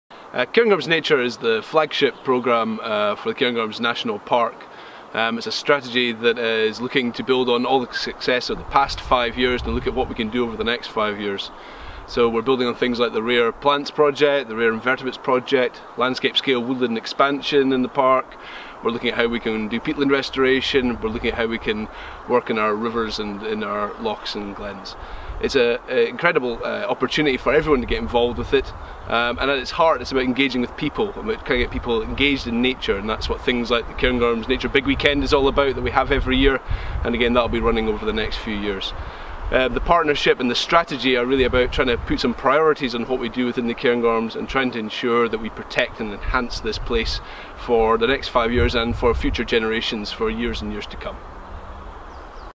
Cairngorms Nature - Grant Moir, Chief Executive, Cairngorms National Park Authority
Grant Moir, Chief Executive of the Cairngorms National Park Authority, reflects on the successes of the last five years of the flagship programme 'Cairngorms Nature' in the Cairngorms National Park, and looks ahead to the next 5 years.